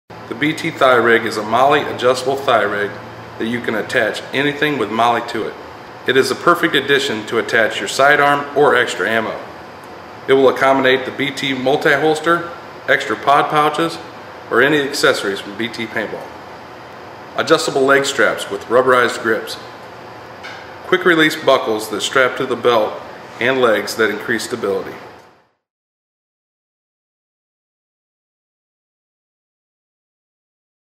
Thigh Rig.flv
BT Thigh Rig
The BT Thigh Rig is a MOLLE adjustable thigh rig that you can attach anything with MOLLE to it.   It is the perfect addition to attach your sidearm or extra ammo.  It will accommodate the BT Multi Holster, extra pod pouches or any of the accessories from BT Paintball.
• Adjustable leg straps with rubberized grips
• Quick release buckles that strap to the belt and legs that increase stability
• Accommodates any of our MOLLE strap accessories
• Available in Woodland Digi only